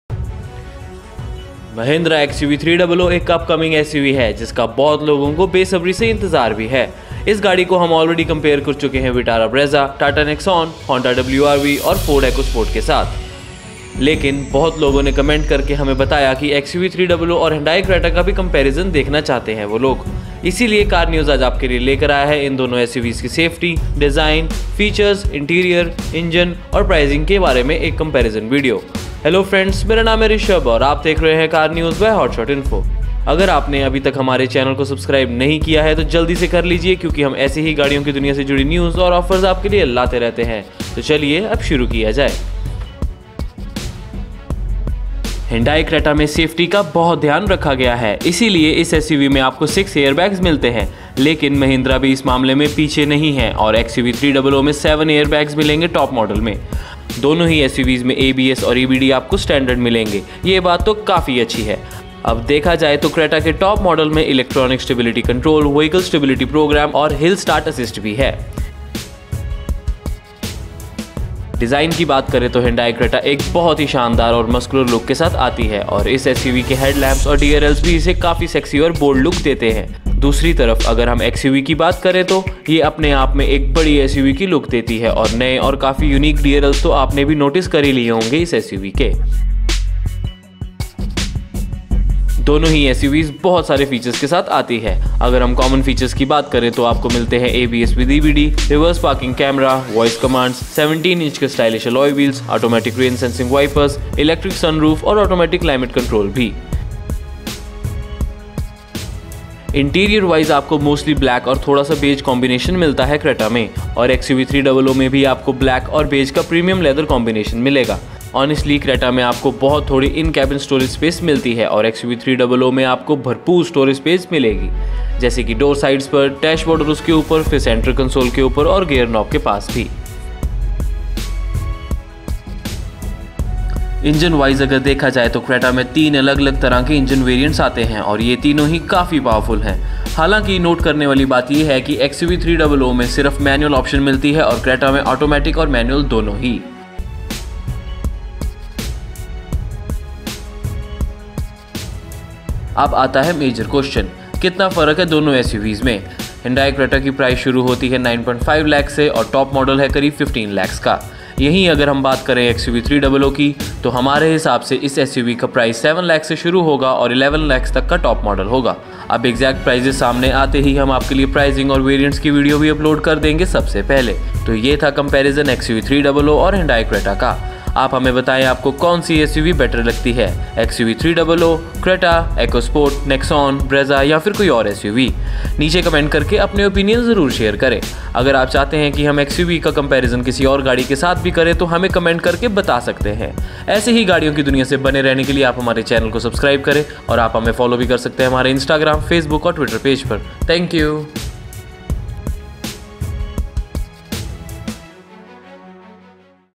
Mahindra XUV300 vs Hyundai Creta - Best SUV 2019 ! Price, Interior, Features aur comparison.
Car News India | Mahindra XUV300 february 2019 me apne rivals ko compete krne ke liye poori trah se tayaar hai. is hindi video me dekhiye Mahindra XUV300 aur Hyundai Creta ka comparison. Jaaniye in dono ke cars price, features, engine, interior, design aur safety ke bare me.